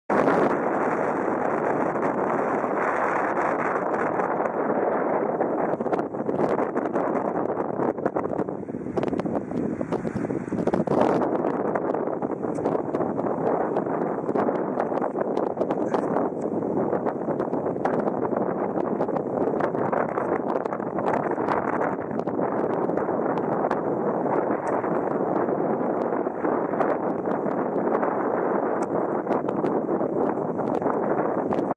WS, Krašci, Ledavsko jezero, severnik
Severni veter, dolgi sunki 20 - 25 knt, lepa dolga špura (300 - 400 m = dve MB šodrci).
Globina 3 - 7 m (govoril s skrbnikom "odtoka")